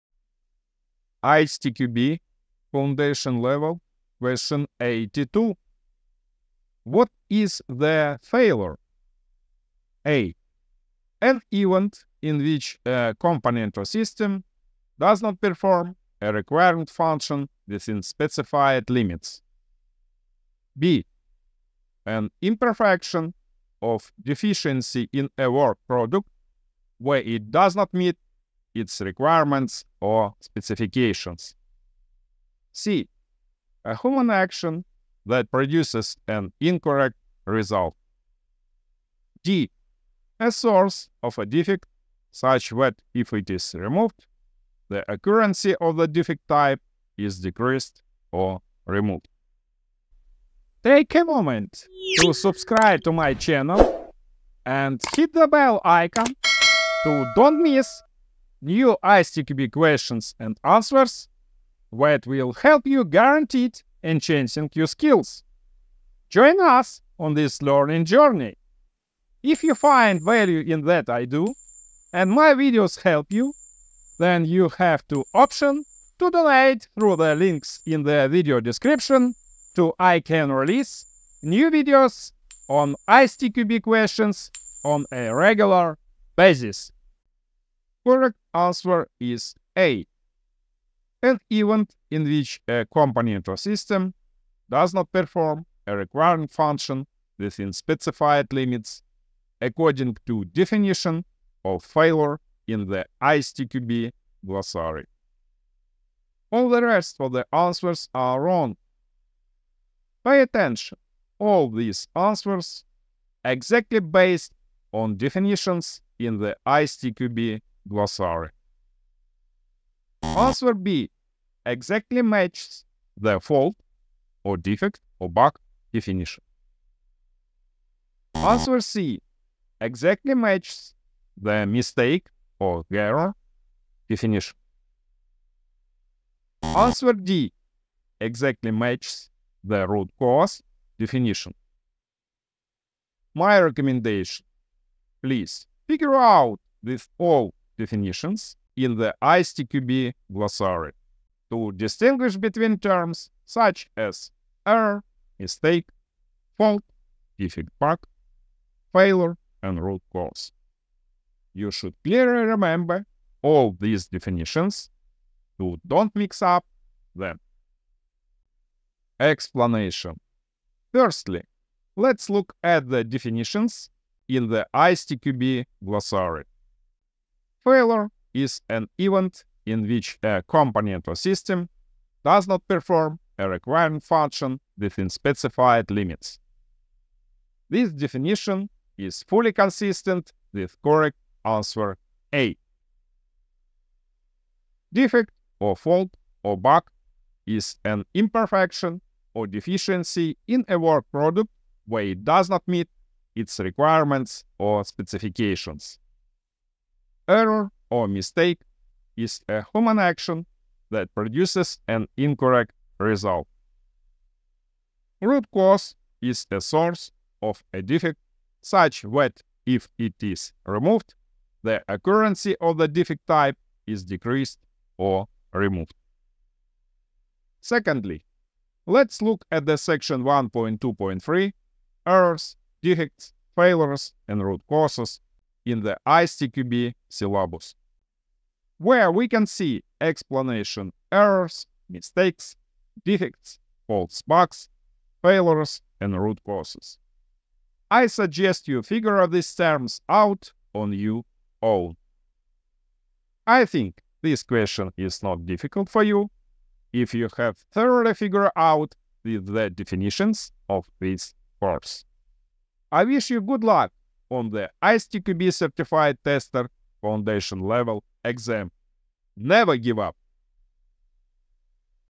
ISTQB. Foundation level v.4.0 (2023). Question #82
ISTQB
Certified Tester Foundation Level v.4.0 (2023)
Question 82.
What is the failure?

00:00 Question.
01:27 Answer.
02:48 Explanation.

This question relates to topic in ISTQB Syllabus:
1.2. Why is Testing Necessary?

Based on:
1. ISTQB CTFL Syllabus v.4.0